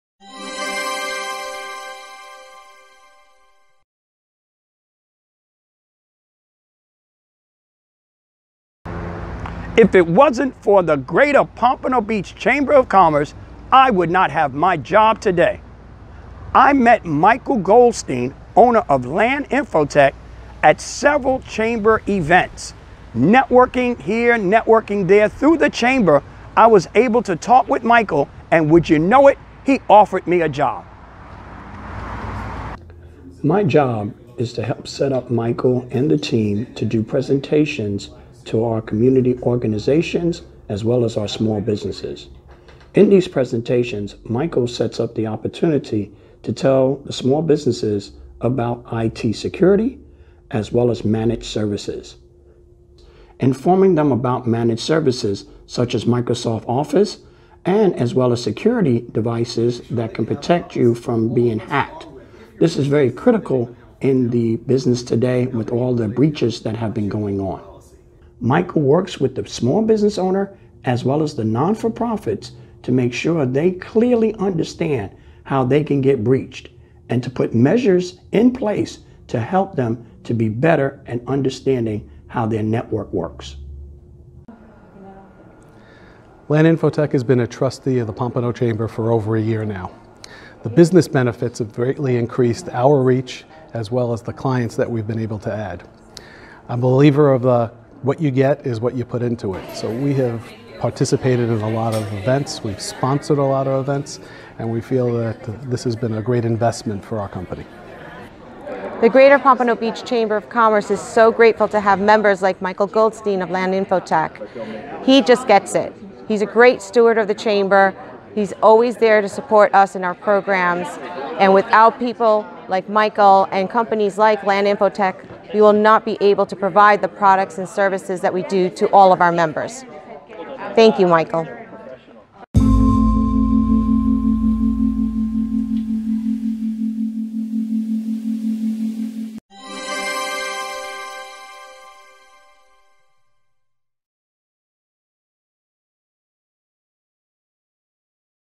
Pompano Beach Chamber salutes | LAN InfoTech
Ceremony saluting Lan InfoTech by the Chamber of Commerce in Pompano Beach, Florida
For more information about Lan Infotech and how you can protect your business,